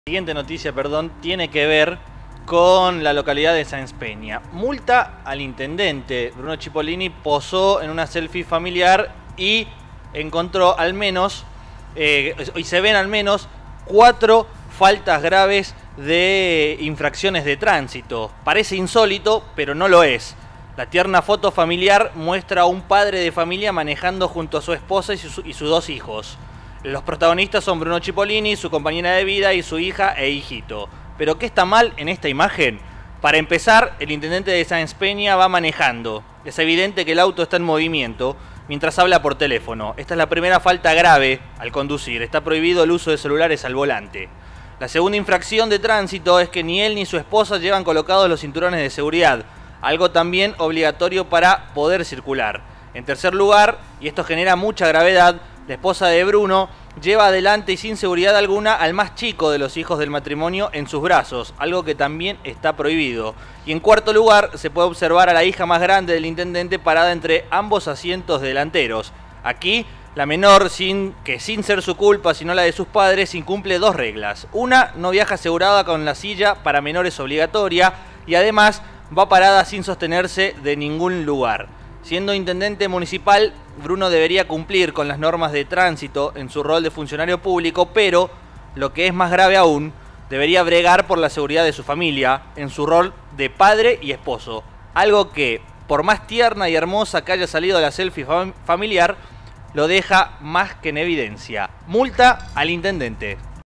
C9 - ¡Multa al Intendente!: Cipolini posó al volante y sumó al menos cuatro infracciones
Una carismática foto familiar dejó en descubierto una muy mala costumbre, no sólo del mandatario municipal – que debería dar el ejemplo- sino de todos los argentinos. En una imagen, el Intendente de Sáenz Peña deja evidenciada, al menos, cuatro faltas graves de tránsito a la hora de circular en un automóvil.